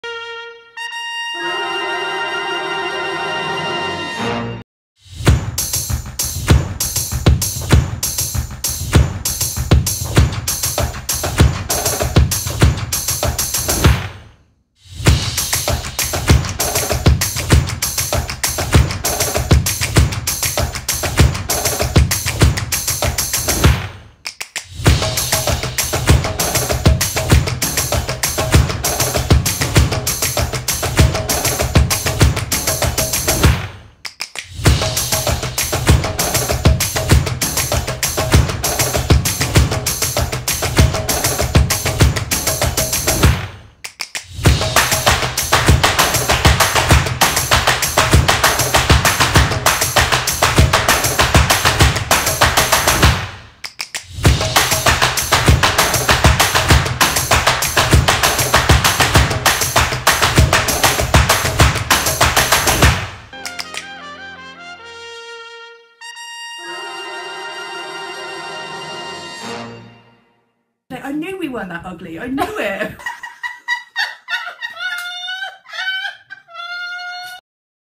Jigsaw time lapse - Blake & Krystle, original 80s Dynasty jigsaw puzzle
One of the 3 original jigsaw puzzle produced in the 1980s as tie-ins to the Prime Time TV Soap Opera Dynasty